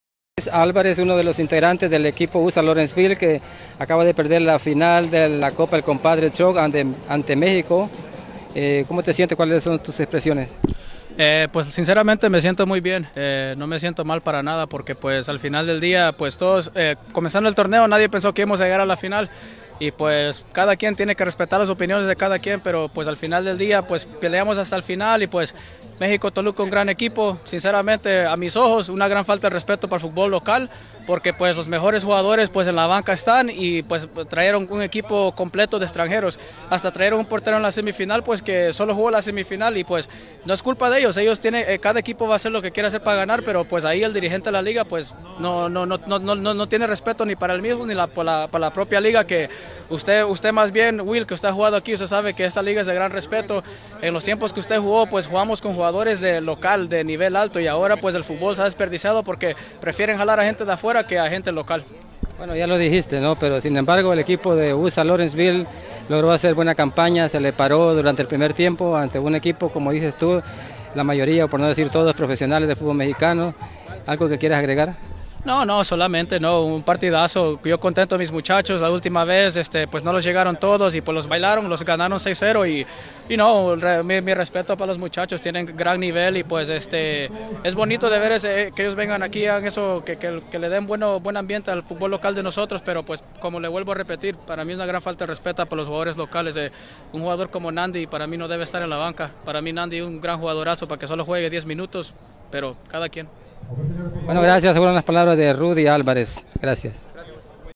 Entrevista a Rudy Álvarez, capitán del equipo U.S.A. Lawrenceville
Entrevista a Rudy Álvarez, capitán del equipo U.S.A. Lawrenceville, sub campeón de la copa el compadre Trucks, categoría Premier